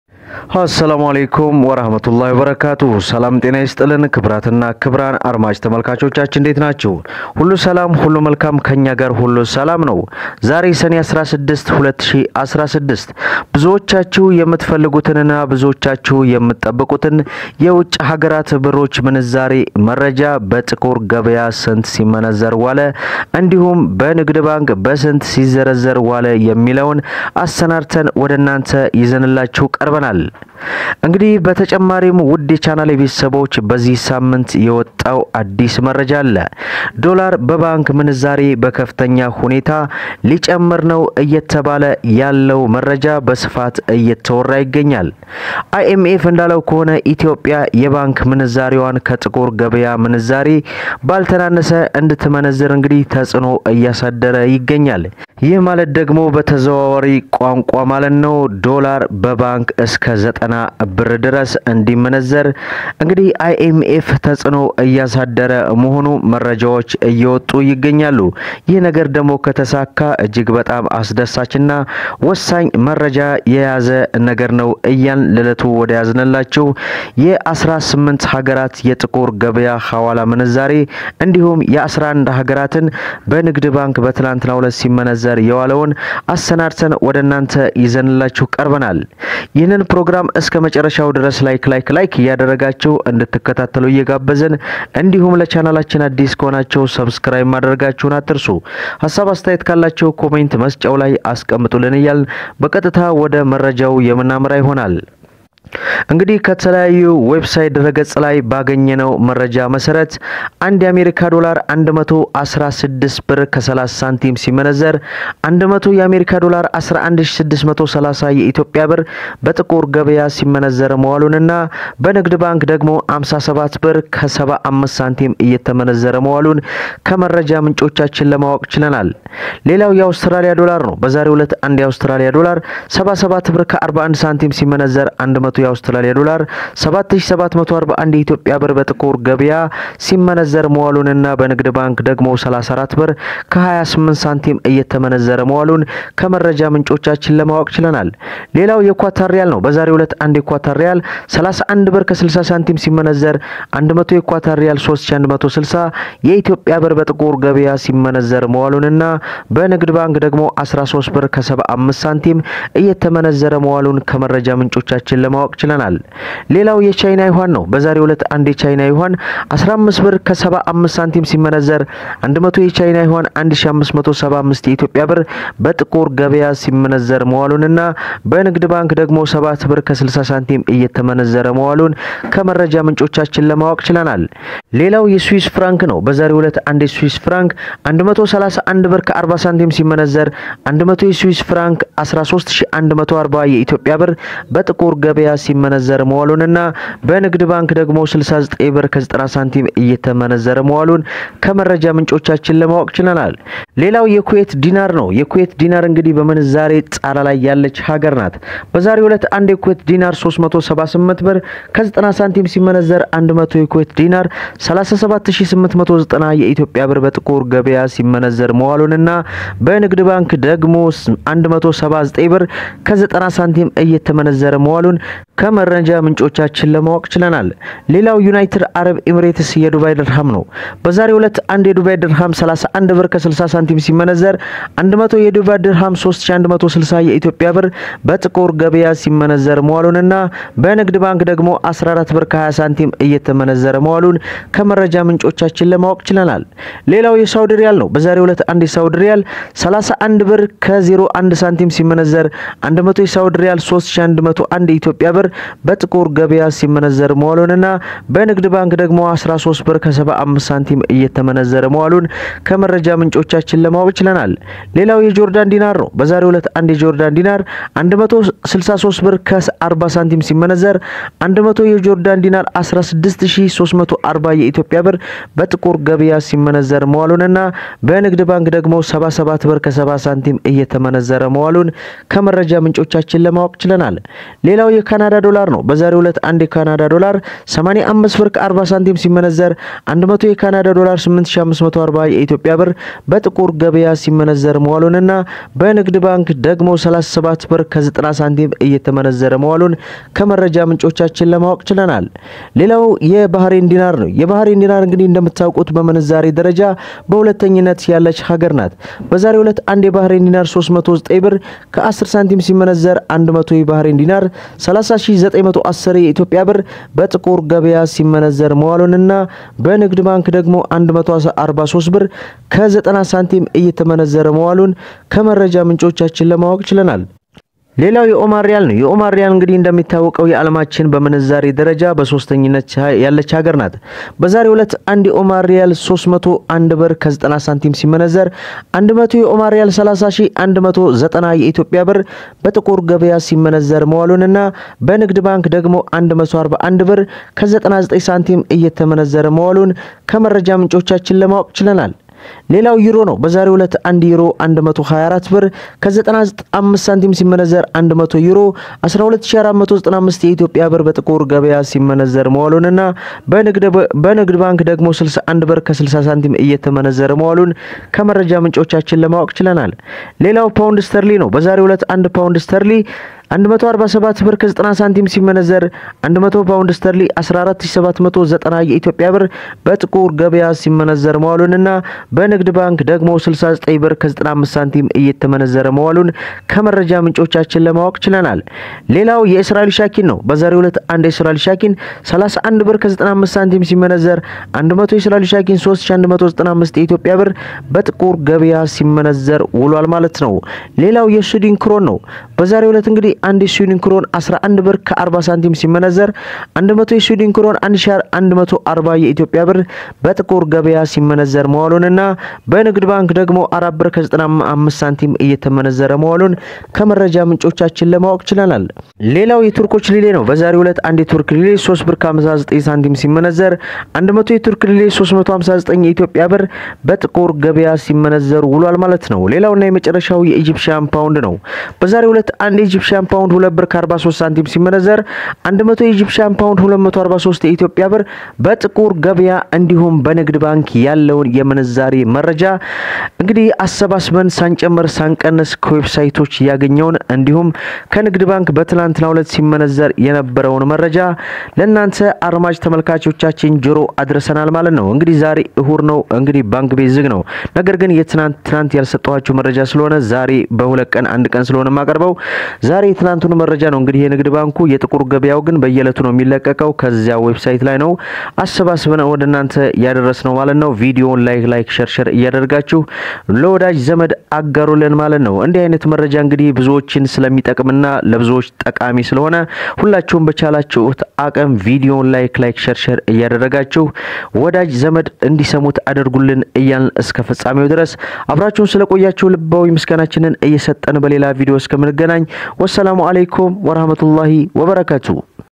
ዛሬ ሰኔ 16/2016 ለማመን ያቅታል የጥቁር ገበያ አበደ | ዶላር በዚህ ሲመነዘር ዋለ ያልሰማችሁ ሰሙ ጉድ | black market in Ethiopia
የሚሸጥ መኪና, የመኪና ዋጋ, መኪና ዋጋ, አባዱላ መኪና, የስራ መኪና, የስራ አባዱላ, አባዱላ, መኪና,